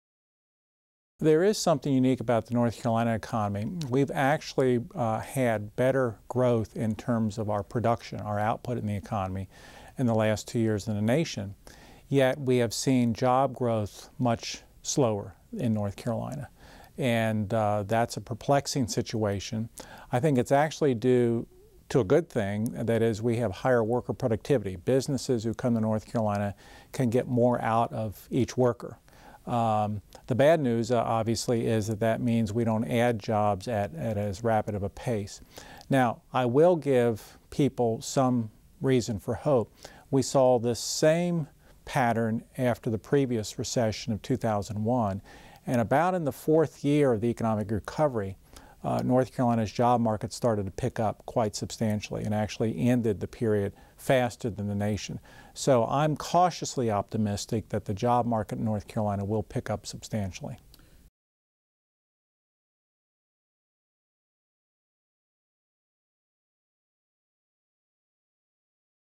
Is there anything unique about the NC economy that will impact the way North Carolinians vote?
Dr. Michael Walden, agricultural and consumer economics, North Carolina State University